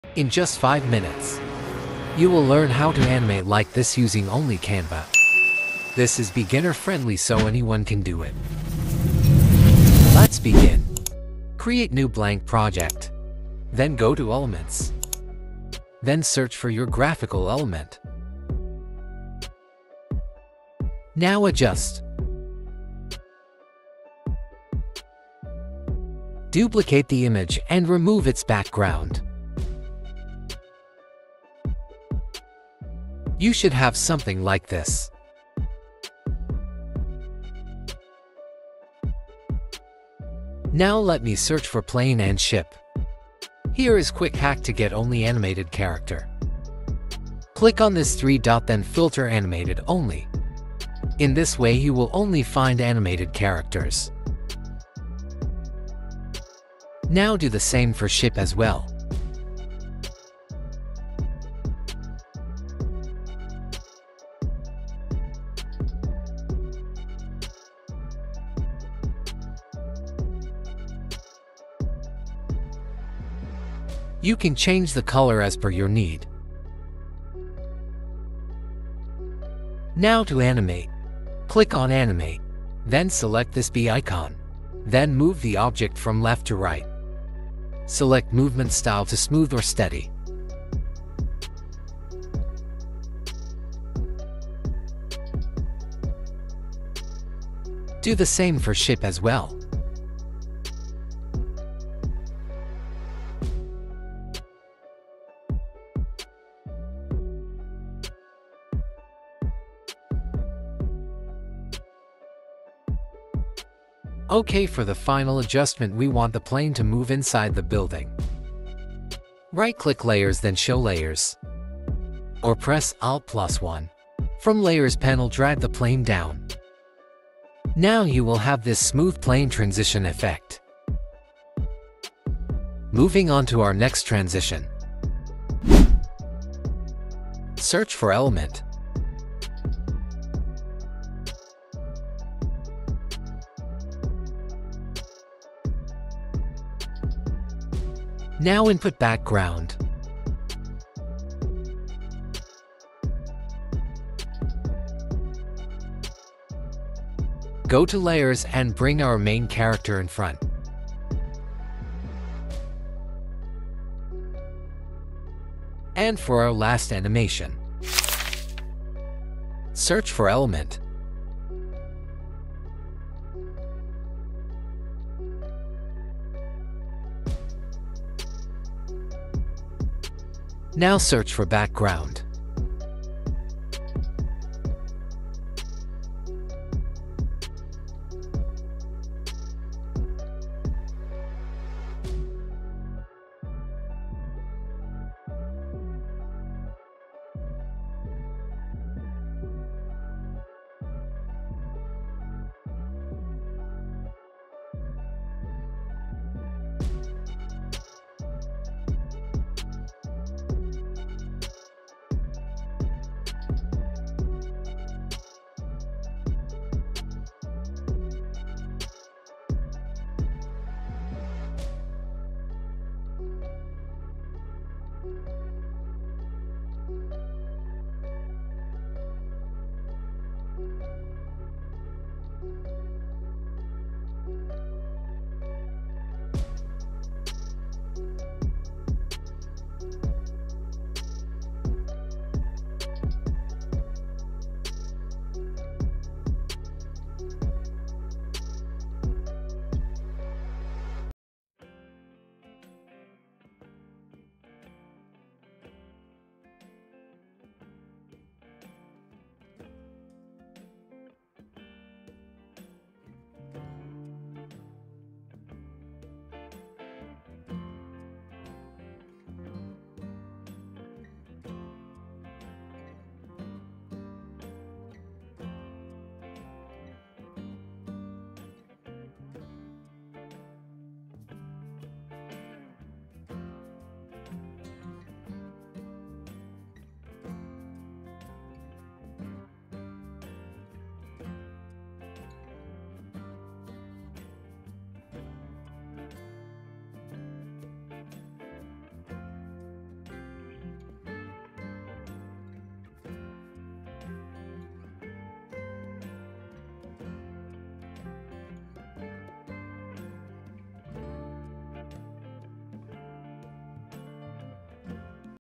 How I Made a Viral Canva Animation in Just 5 Minutes! | Easy Canva Tutorial
Want to create stunning Canva animation videos in just 5 minutes? This tutorial will show you how to make high-quality animations without any design skills! Whether you're a beginner or an expert, Canva makes it easy to create eye-catching animated content for YouTube, Instagram, and TikTok. I'll walk you through step-by-step, revealing hidden tricks and pro-level hacks to level up your video editing game.

Discover how to add motion effects, customize animations, and bring your ideas to life effortlessly. You won’t need expensive software—just Canva! If you're ready to impress your audience with smooth and professional-looking animations, hit play and start creating today!

0:00 Intro
0:13 Basic Canva Animation
1:50 Plane inside building
2:31 Dog animation
2:53 Space